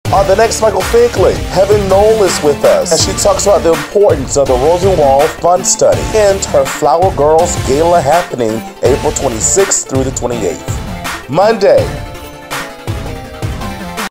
PROMO 3/25/2024
Monday! Helen Noel talks about the importance of the Rosenwald Fund Study and the Flower Gurlz Gala happening April 26-28, 2024. Don’t miss it!